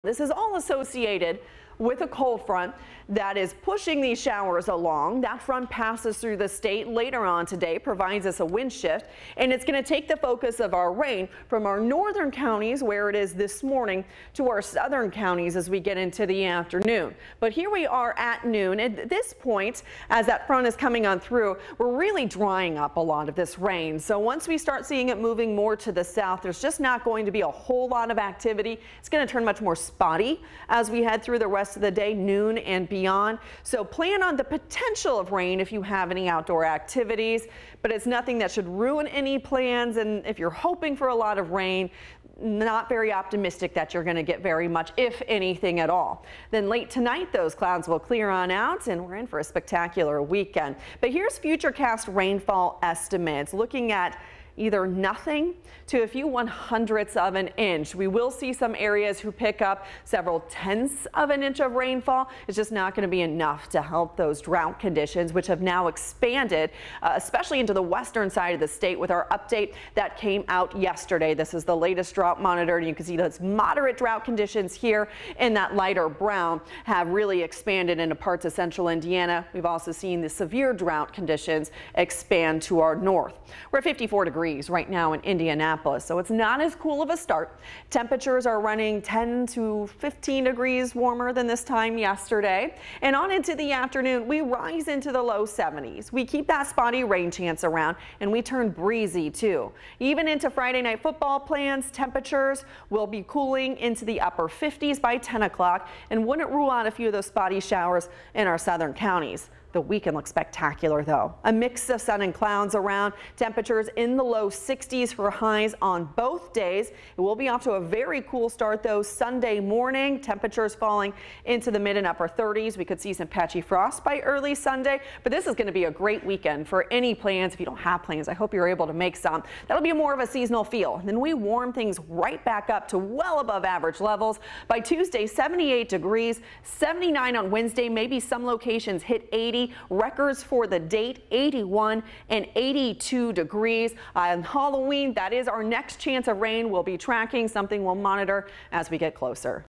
Rain chances linger Friday; a fantastic weekend
Rain chances will linger through the day on Friday, although overall totals for most won't amount to much.